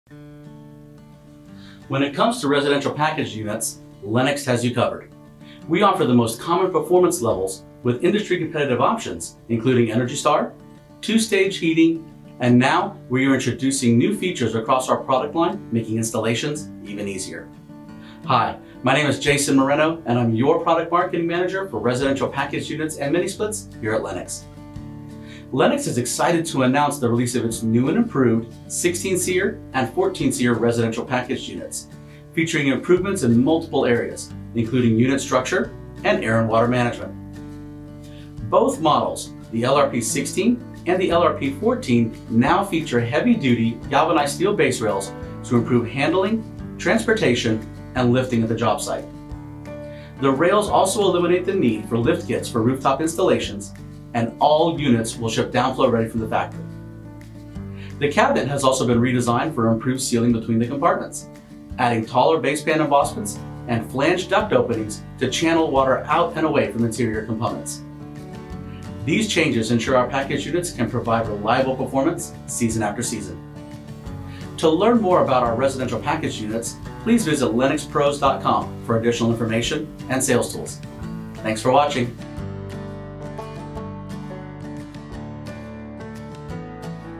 Lennox Residential Package Units - Update
Lennox is excited to announce the release of its new and improved 16 SEER and 14 SEER Residential Packaged Units, featuring improvements in multiple areas including unit structure and air and water management.